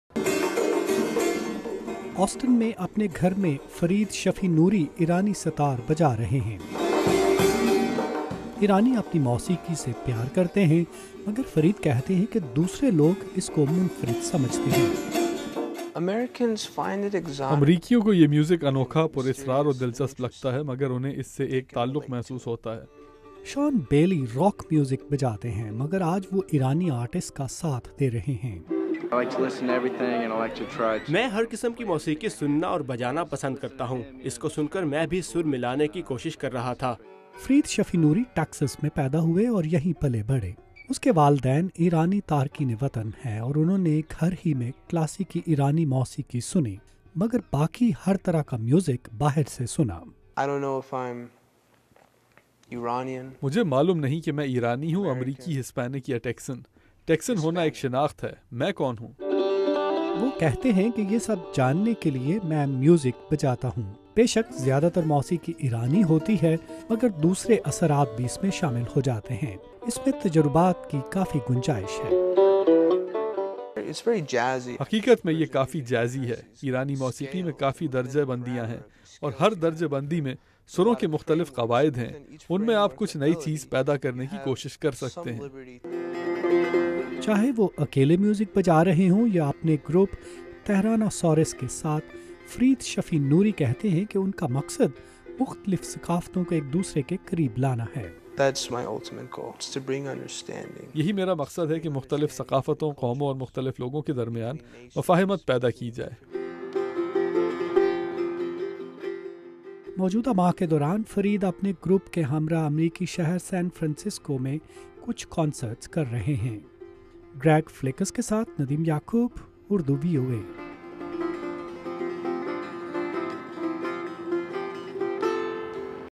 Iranian-American Musician
As VOA's Greg Flakus reports from Austin, Texas, Iranian-American musician and songwriter Fared Shafinury is developing a new sound, combining Persian classical music and American styles.